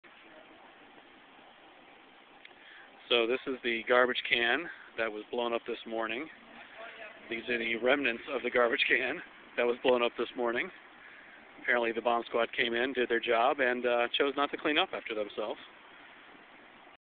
Embedded in Hayward
On 9-21-07, the bomb squad of the Alameda County Sheriff's Department detonated a garbage can that was positively identified by their sniffer dogs as containing "something."  This gritty, raw footage shows the aftermath.  Who knew Hayward and Bagdad had so much in common?